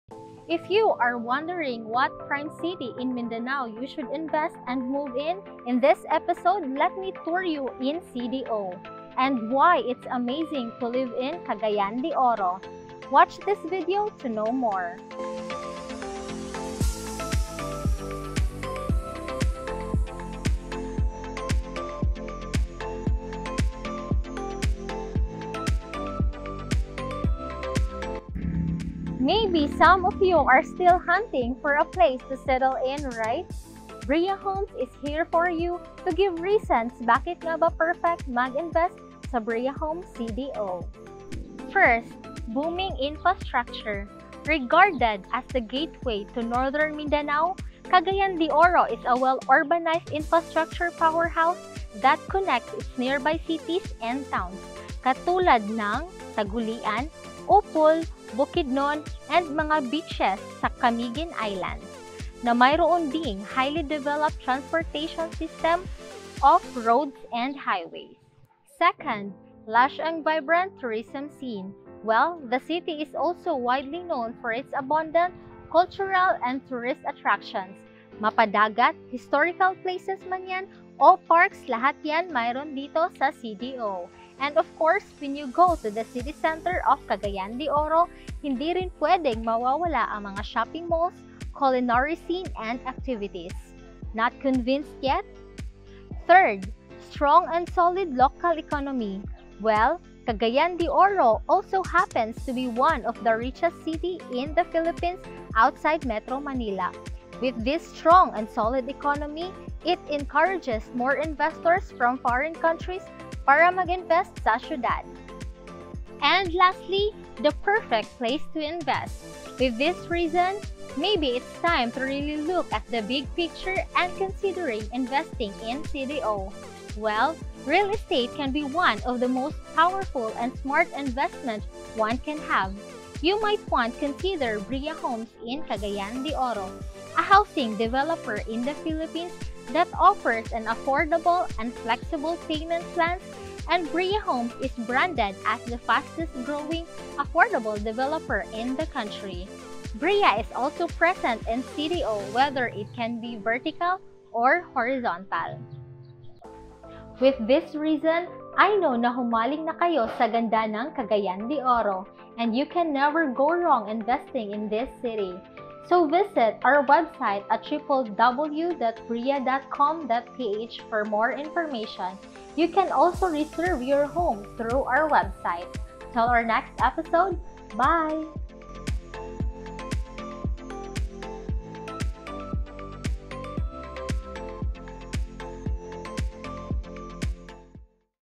Bria Talks 18 | What it's like living in Cagayan De Oro
Cagayan De Oro is a beautiful and fascinating city in the southern Philippines, but what is it like living in Cagayan De Oro As? Today’s ultimate guide, you'll know some factors that will hopefully nudge you to invest in the glowing city, and in turn, help you experience for yourself why Cagayan de Oro is the perfect investment for you.